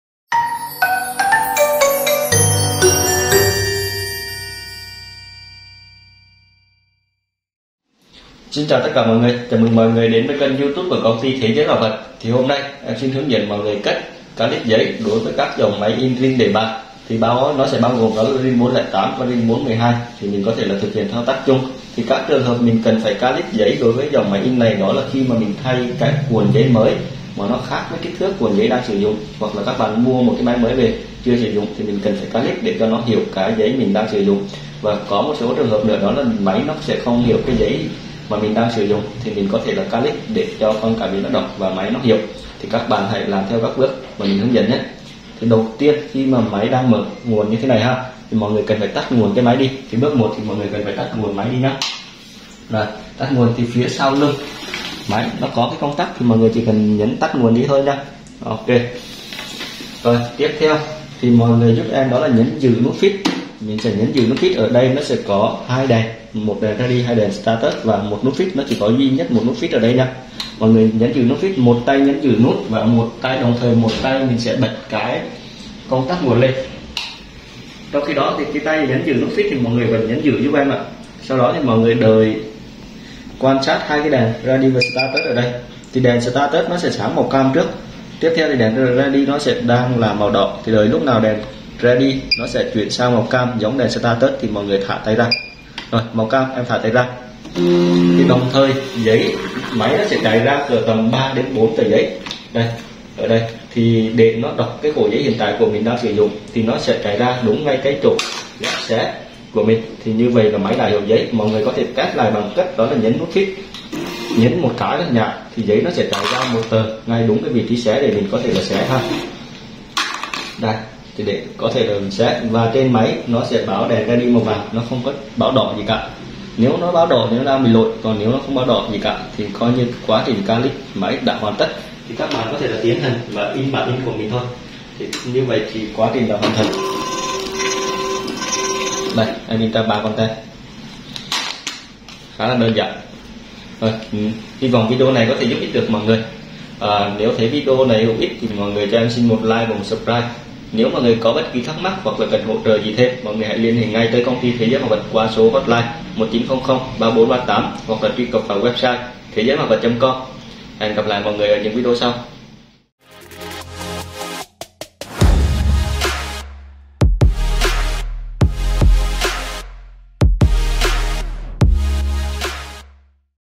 Calibration máy in mã vạch để bàn RING (RING 408PEI+, RING 412PEI+)
🎀 Hướng dẫn Calibration cho máy in mã vạch để bàn RING (Autonics) với các bước thực hiện như sau: (RING 408PEI+, RING 412PEI+):
- 00:46 : Tắt nguồn máy in.
- 01:02 : Vừa đè nút Feed trên nắp máy, vừa bật nguồn.
- 01:19 : Vẫn tiếp tục đè giữ nút Feed cho đến khi 2 đèn LED chỉ báo cùng hiện màu cam thì thả tay ra (quá trình này kéo dài khoảng 5 giây, trước đèn cam sẽ là đèn đỏ).
Sau khi đã thả tay, máy tự động chạy ra tầm 3,4 con tem trắng rồi dừng lại. Lúc này bạn đã thực hiện Calibration thành công.

👉 Bạn xem có thể tham khảo chi tiết hơn qua tài liệu